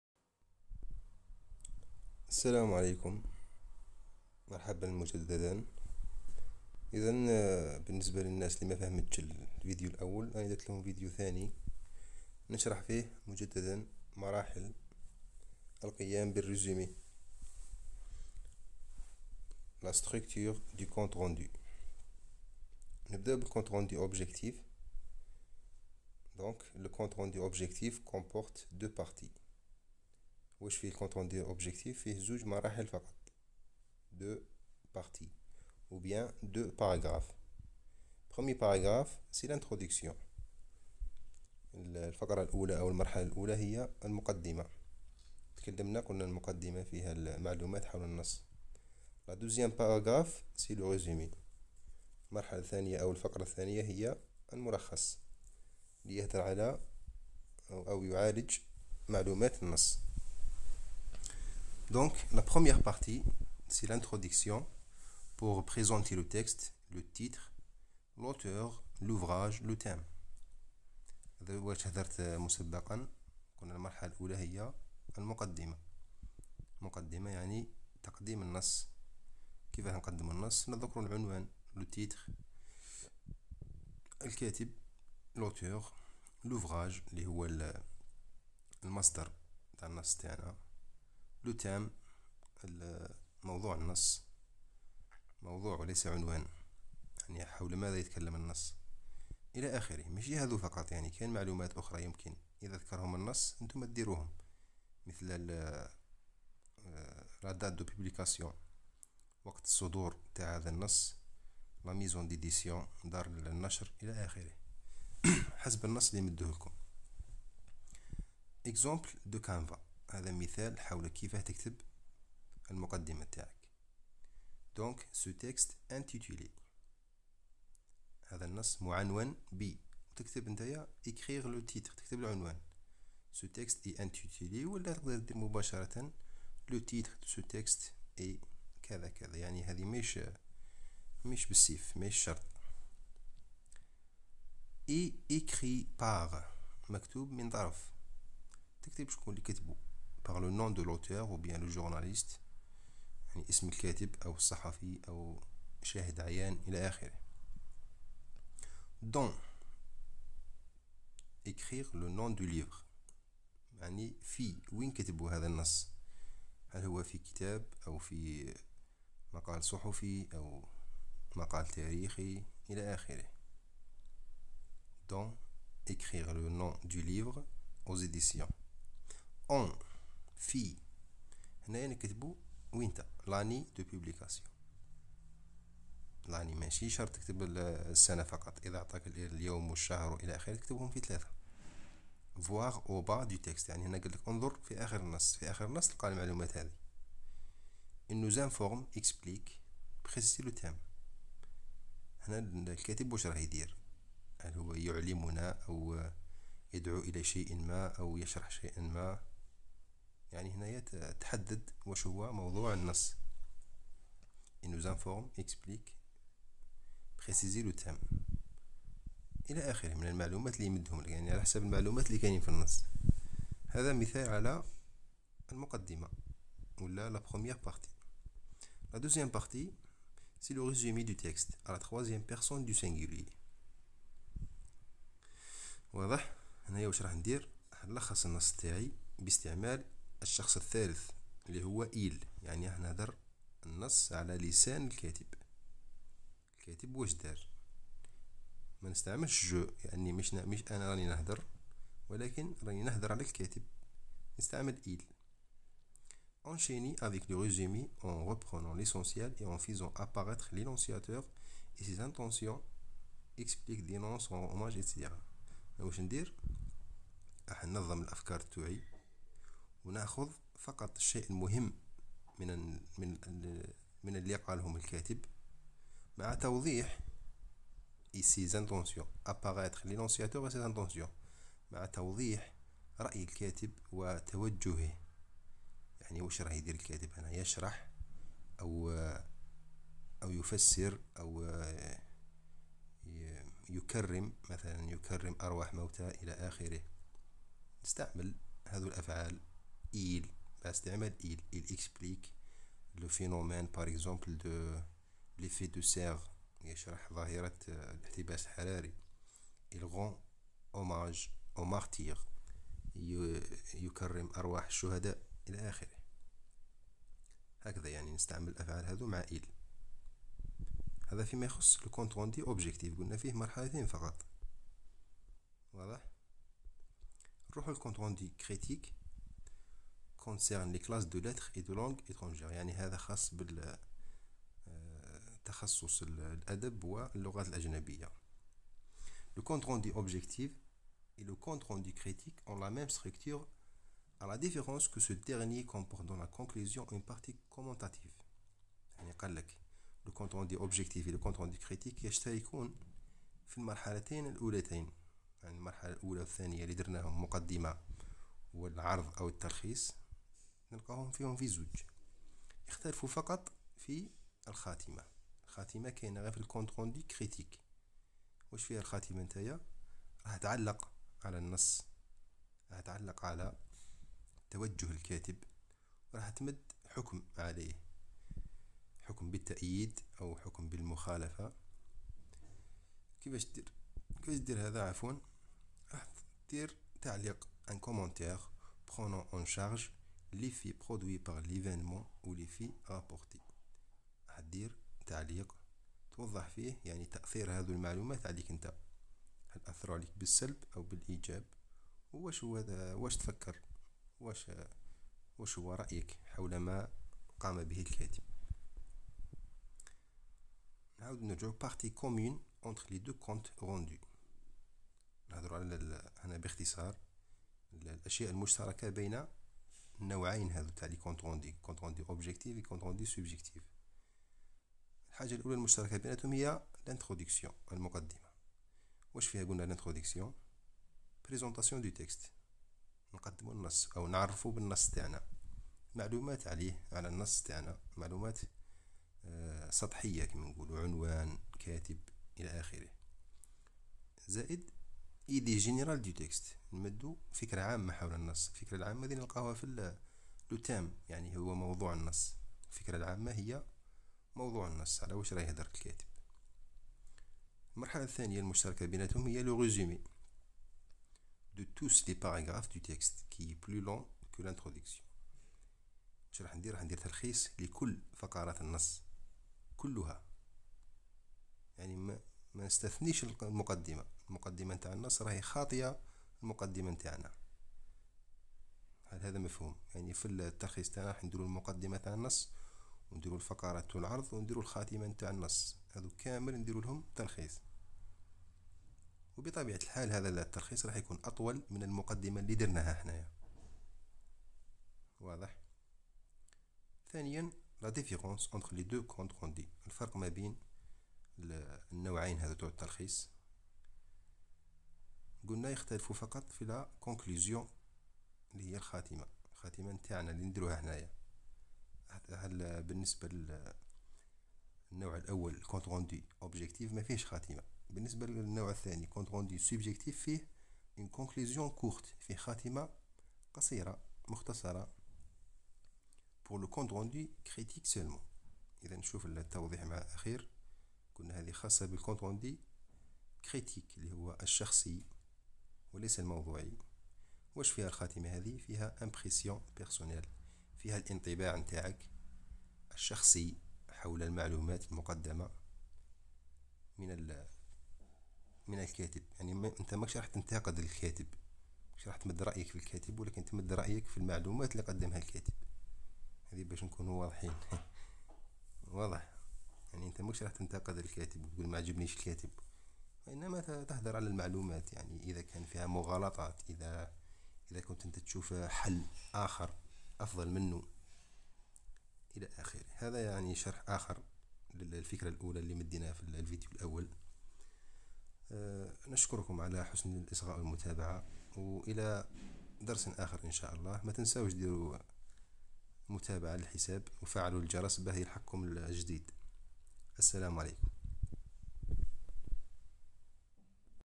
كيف أنجح في كتابة تلخيص موضوعي أو شخصي ( إعادة شرح) LE COMPTE RENDU
في هذا الفيديو قمت بشرح جديد للفيديو السابق حول الفرق وكيفية تحرير ملخص سواء كان ذاتي compte rendu objectif
أو شخصي compte rendu critique